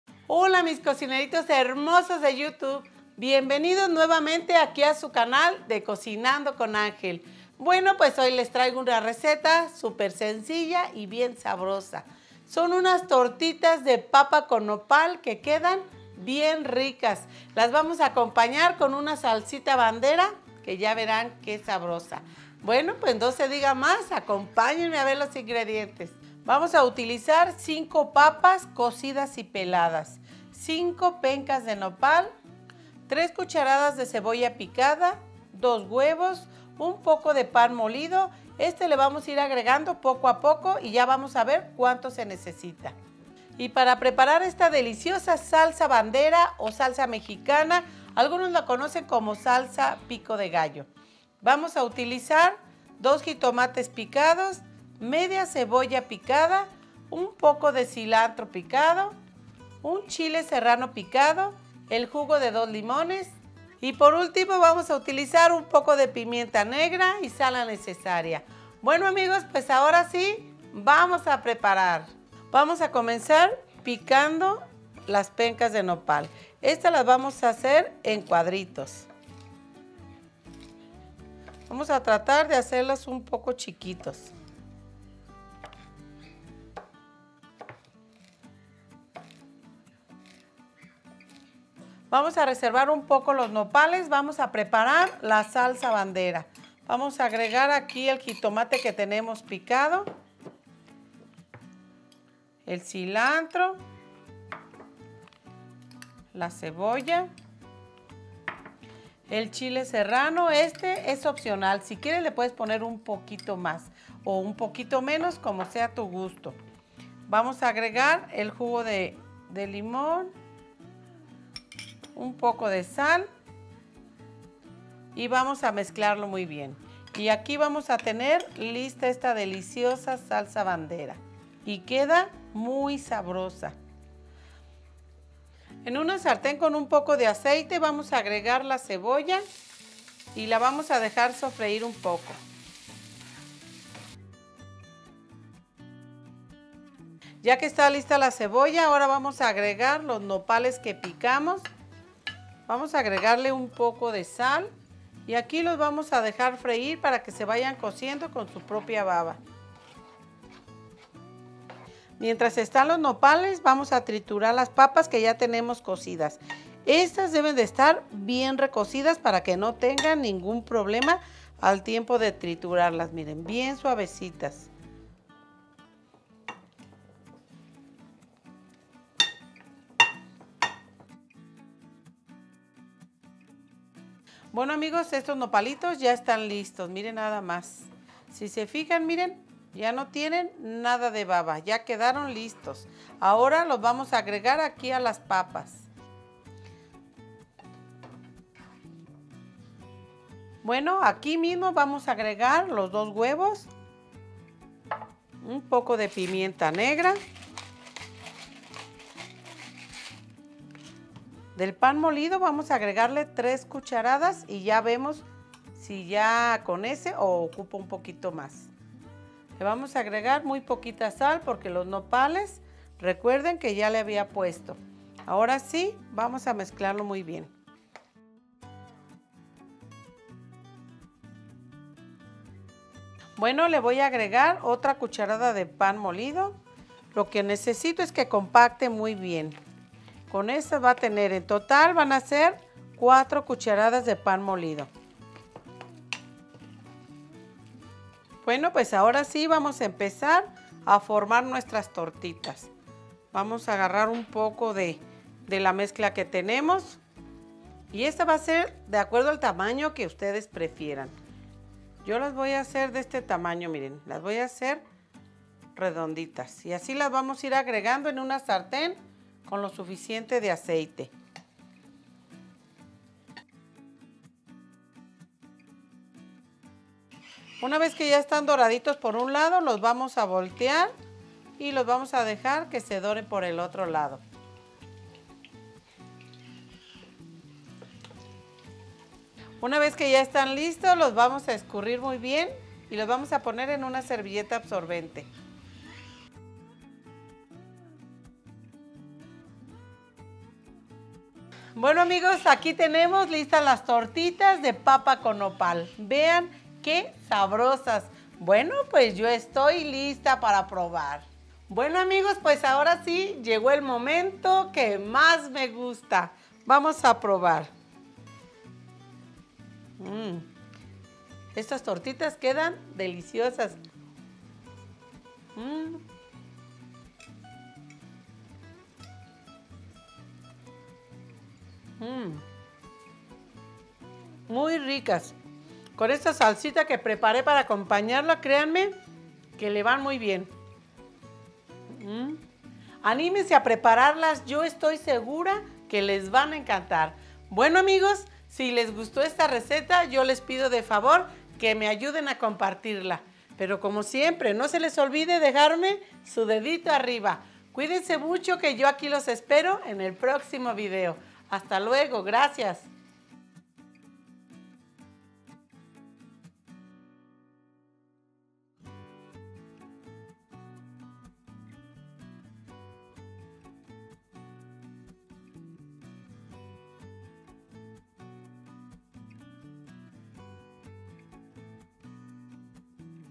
Tortitas de Nopal con Papa Fáciles
Tortitas de Nopal con Papa Fáciles, como hacer tortitas de papa muy sabrosas y fáciles de preparar en Cocinando con Angel. Recetas caseras y Fáciles.

*INGREDIENTES:
-5 Papas Cocidas y peladas.
-5 Pencas de nopal.
-3 Cdas. de cebolla picada.
-2 Huevos.
-Pan molido.
-2 Jitomates picados.
-1/2 pieza de cebolla picada.
-Cilantro picado.
-1 Chile serrano picado.
-2 Limonesd (el jugo).
-Pimienta negra.
-Sal.